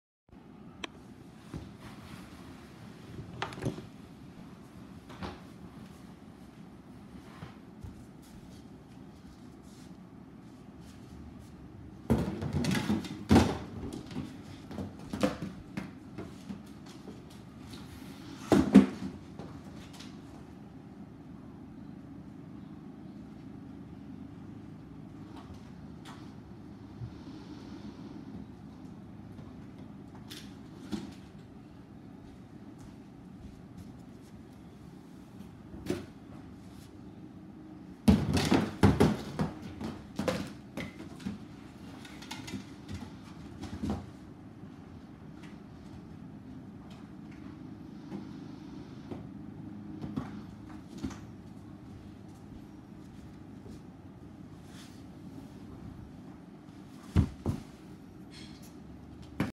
DIY cardboard bowling alley (MOST VIEWED VIDEO!)
I made all this all by myself I put lights on the alley to make it look like A real alley and I used a string pinsetter that I made and I used a set of toy plastic pins and a bowlercade bowling ball